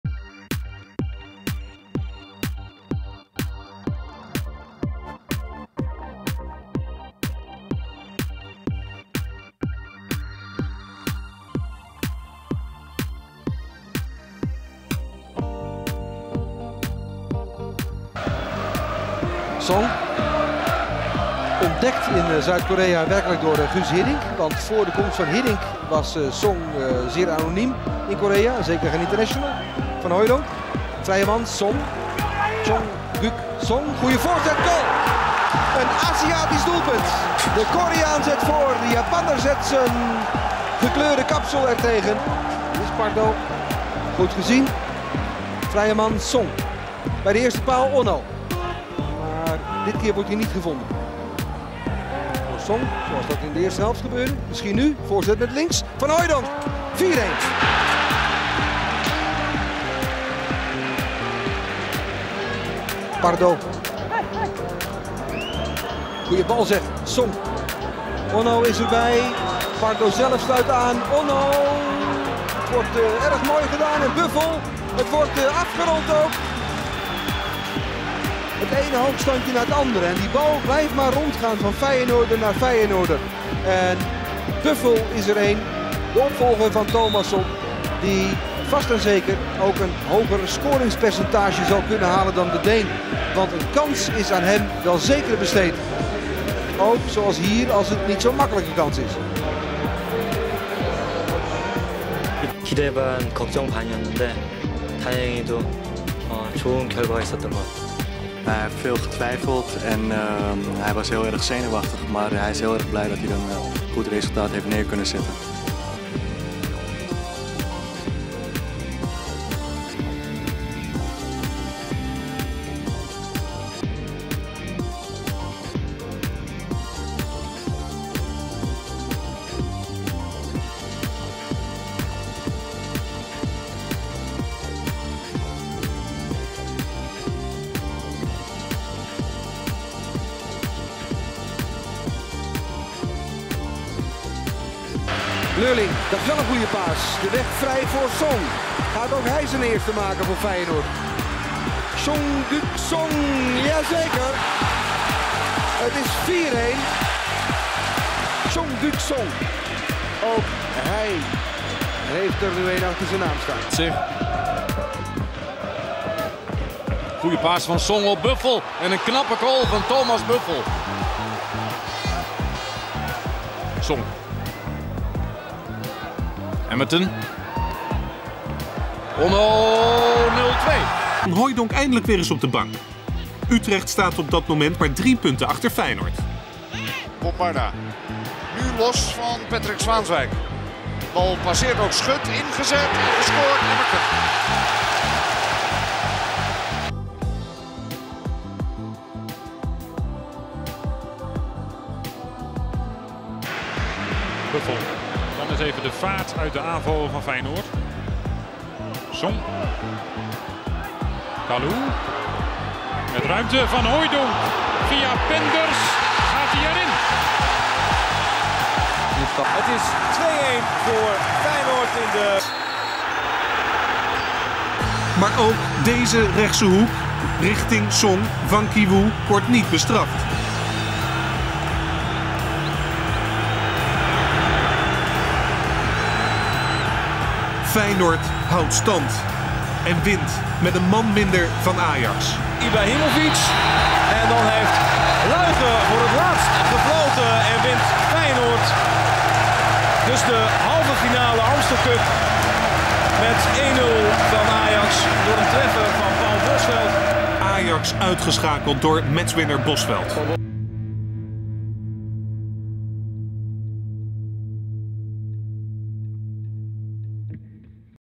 송종국, 데뷔 첫 시즌 페예노르트 활약 모음
The highlight of Song Chong-gug, the best right fullback in Korean football history